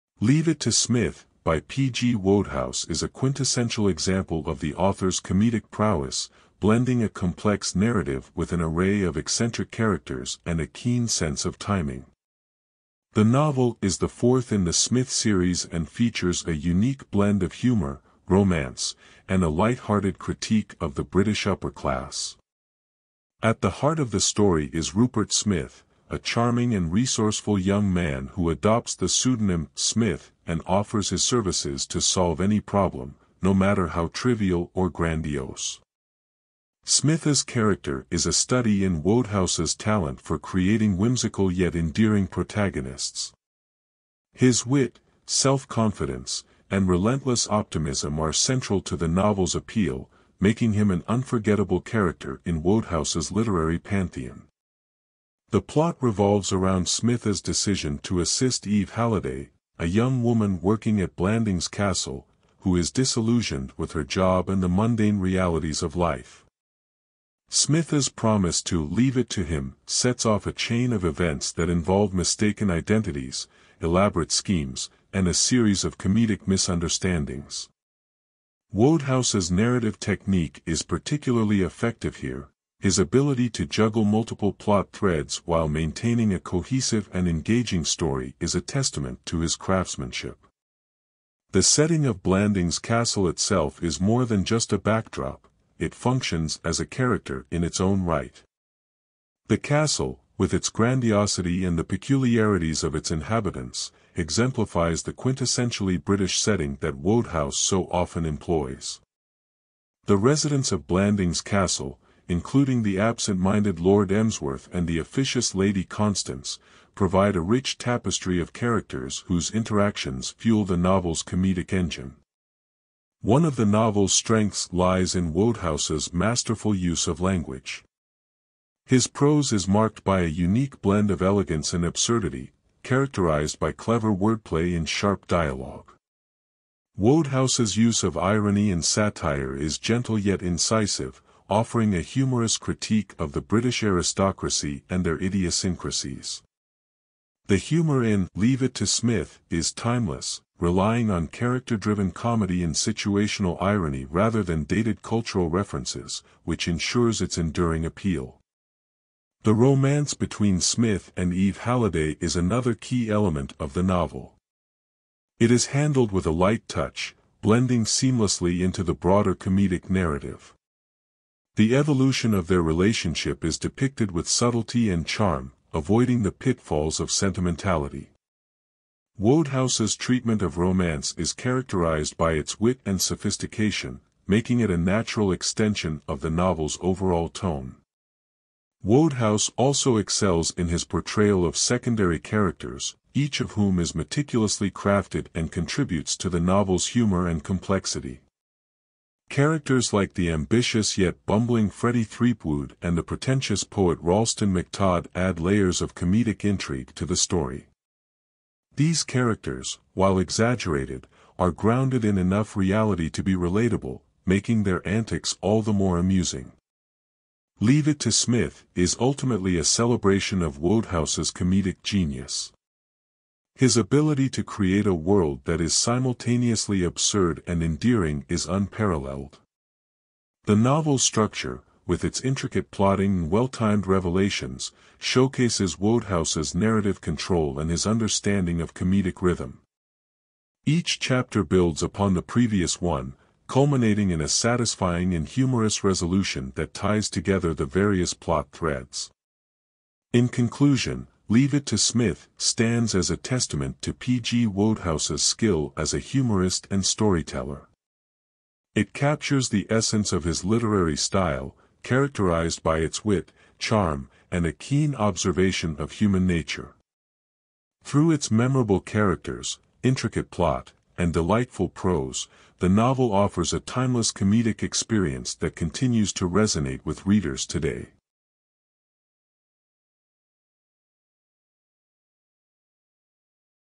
"Leave It to Psmith (Psmith, #4)" By P.G. Wodehouse
"Leave It to Psmith" by P.G. Wodehouse is a quintessential example of the author's comedic prowess, blending a complex narrative with an array of eccentric characters and a keen sense of timing. The novel is the fourth in the Psmith series and features a unique blend of humor, romance, and a light-hearted critique of the British upper class.
At the heart of the story is Rupert Psmith, a charming and resourceful young man who adopts the pseudonym “Psmith” and offers his services to solve any problem, no matter how trivial or grandiose. Psmith's character is a study in Wodehouse's talent for creating whimsical yet endearing protagonists. His wit, self-confidence, and relentless optimism are central to the novel's appeal, making him an unforgettable character in Wodehouse's literary pantheon.
The plot revolves around Psmith's decision to assist Eve Halliday, a young woman working at Blandings Castle, who is disillusioned with her job and the mundane realities of life. Psmith's promise to "leave it to him" sets off a chain of events that involve mistaken identities, elaborate schemes, and a series of comedic misunderstandings. Wodehouse’s narrative technique is particularly effective here; his ability to juggle multiple plot threads while maintaining a cohesive and engaging story is a testament to his craftsmanship.
The setting of Blandings Castle itself is more than just a backdrop; it functions as a character in its own right. The castle, with its grandiosity and the peculiarities of its inhabitants, exemplifies the quintessentially British setting that Wodehouse so often employs. The residents of Blandings Castle, including the absent-minded Lord Emsworth and the officious Lady Constance, provide a rich tapestry of characters whose interactions fuel the novel's comedic engine.
One of the novel’s strengths lies in Wodehouse's masterful use of language. His prose is marked by a unique blend of elegance and absurdity, characterized by clever wordplay and sharp dialogue. Wodehouse’s use of irony and satire is gentle yet incisive, offering a humorous critique of the British aristocracy and their idiosyncrasies. The humor in "Leave It to Psmith" is timeless, relying on character-driven comedy and situational irony rather than dated cultural references, which ensures its enduring appeal.
The romance between Psmith and Eve Halliday is another key element of the novel. It is handled with a light touch, blending seamlessly into the broader comedic narrative. The evolution of their relationship is depicted with subtlety and charm, avoiding the pitfalls of sentimentality. Wodehouse's treatment of romance is characterized by its wit and sophistication, making it a natural extension of the novel's overall tone.
Wodehouse also excels in his portrayal of secondary characters, each of whom is meticulously crafted and contributes to the novel’s humor and complexity. Characters like the ambitious yet bumbling Freddie Threepwood and the pretentious poet Ralston McTodd add layers of comedic intrigue to the story. These characters, while exaggerated, are grounded in enough reality to be relatable, making their antics all the more amusing.
"Leave It to Psmith" is ultimately a celebration of Wodehouse's comedic genius. His ability to create a world that is simultaneously absurd and endearing is unparalleled. The novel’s structure, with its intricate plotting and well-timed revelations, showcases Wodehouse's narrative control and his understanding of comedic rhythm. Each chapter builds upon the previous one, culminating in a satisfying and humorous resolution that ties together the various plot threads.
In conclusion, "Leave It to Psmith" stands as a testament to P.G. Wodehouse's skill as a humorist and storyteller. It captures the essence of his literary style, characterized by its wit, charm, and a keen observation of human nature. Through its memorable characters, intricate plot, and delightful prose, the novel offers a timeless comedic experience that continues to resonate with readers today.